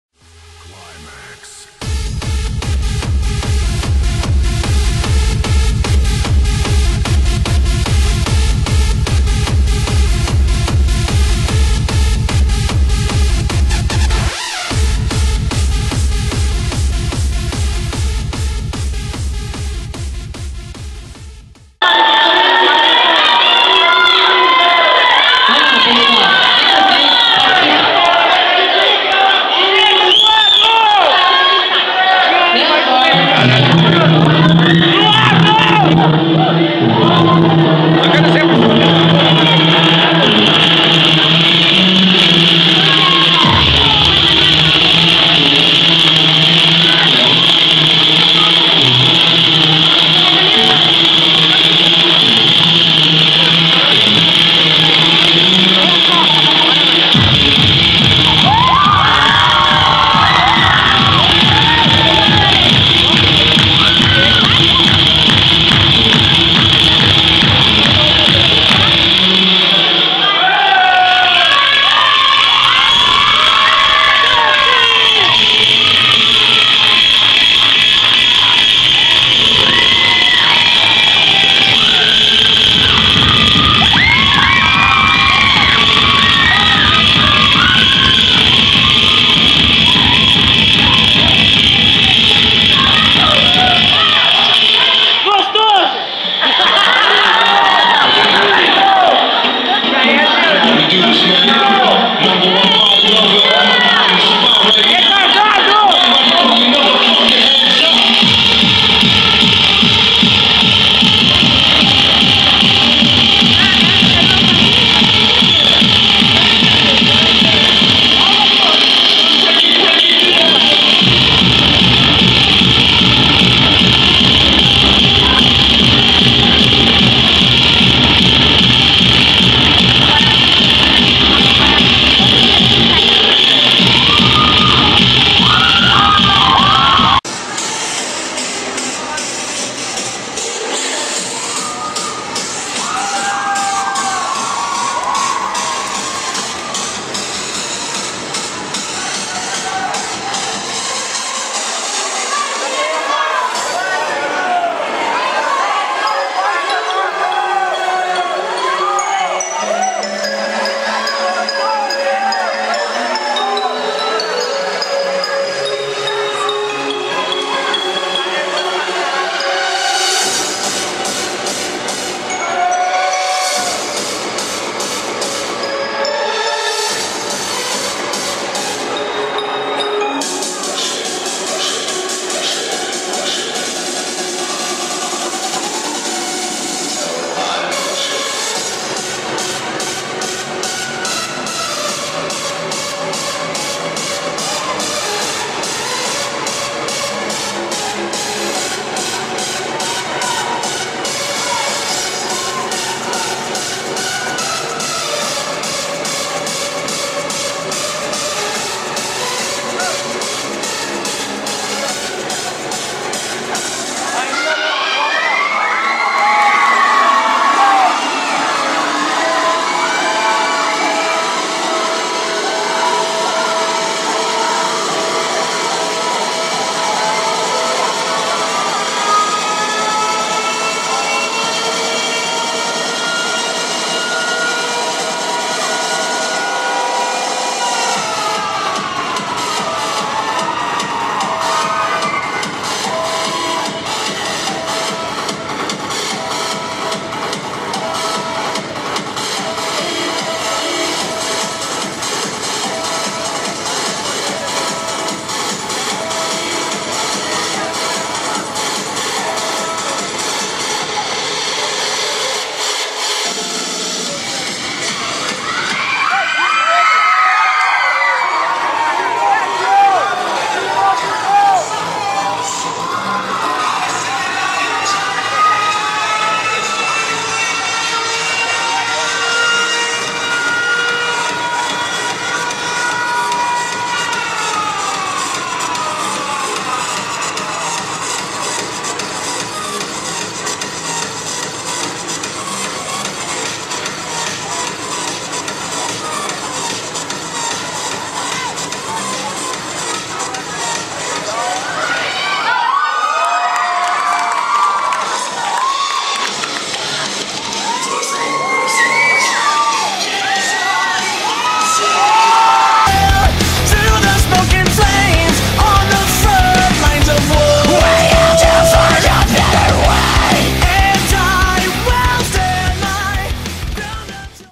Apresentação de Melb. Shuffle OWNED ! |E.S| and |HSQ
Apresentação WIN, fico meio estranho a coreografia porque agente começo a treinar na semana d Show de talentos, senão tinha ficado muito mais foda, e pra quem nao sabe o que é C.P.A é  "Coronel Pedro Arbues", Comment And Rate plx *-*